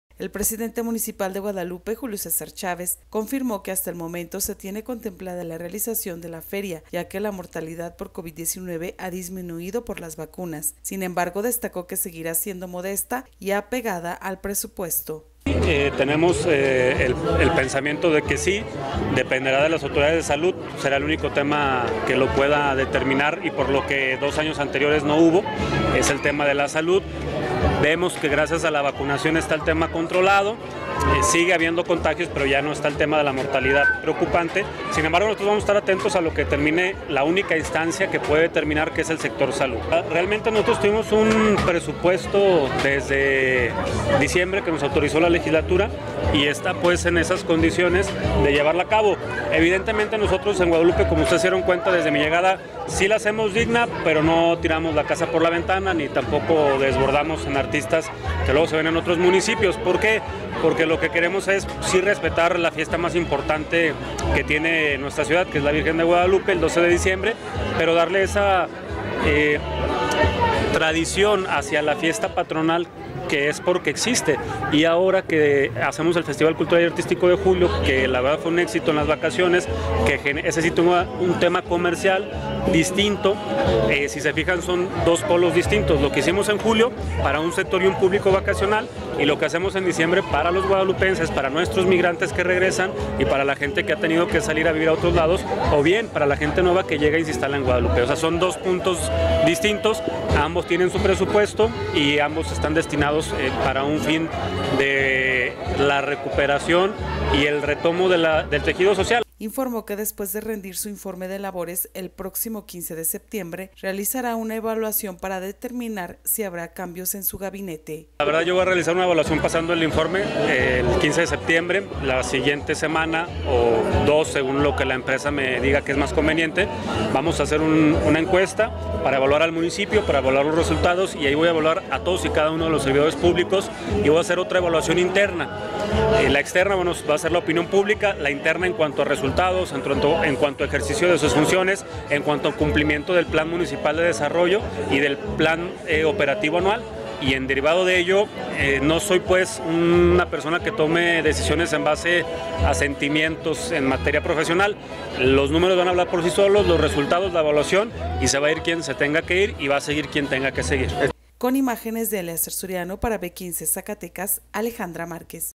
Sí habrá Feria en Guadalupe modesta y apegada al presupuesto: Julio César Chávez.
Zacatecas, Zacatecas. –El presidente municipal de Guadalupe Julio César Chávez confirmó que hasta el momento, se tiene contemplada la realización de la feria, ya que la mortalidad por Covid-19 ha disminuido por las vacunas, sin embargo, destacó que seguirá siendo modesta y apegada al presupuesto.

Informó que después de rendir su informe de labores el próximo 15 de septiembre, realizará una evaluación para determinar si habrá cambios en su gabinete.